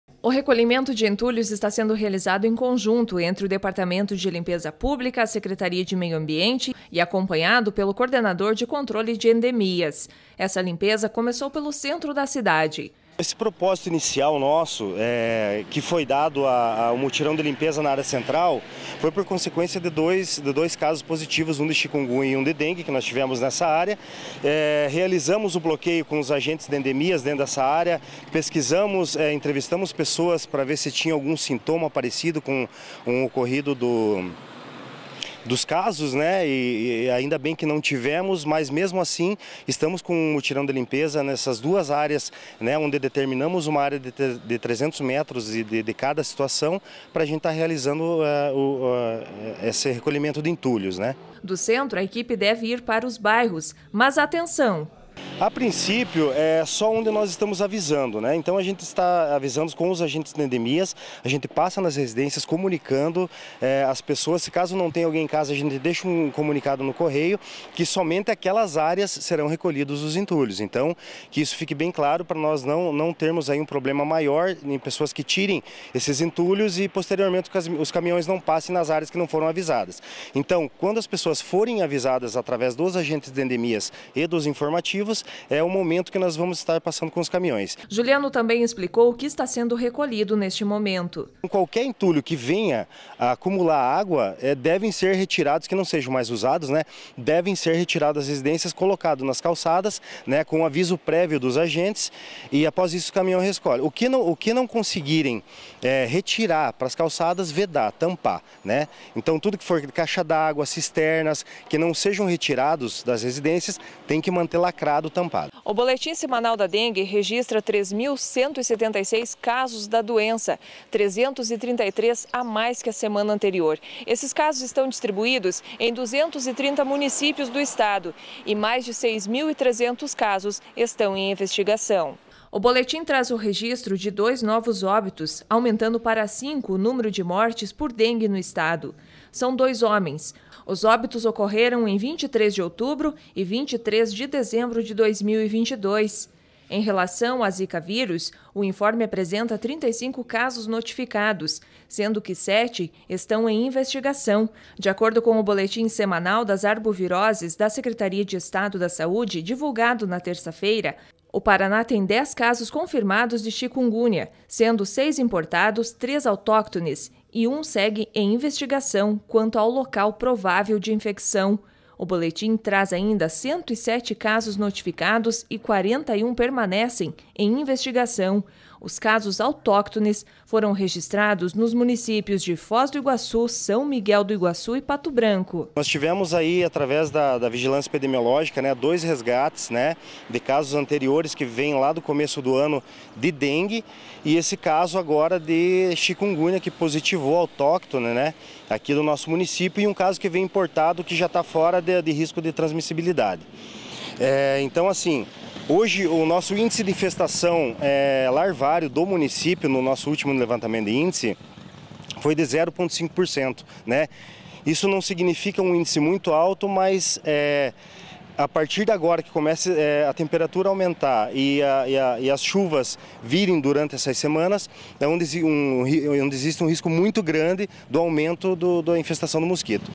Pato Branco realiza mutirão de limpeza no centro da cidade
Saúde: Mutirão de limpeza no centro de Pato Branco, é uma das estratégias para tentar evitar a proliferação do mosquito transmissor da dengue.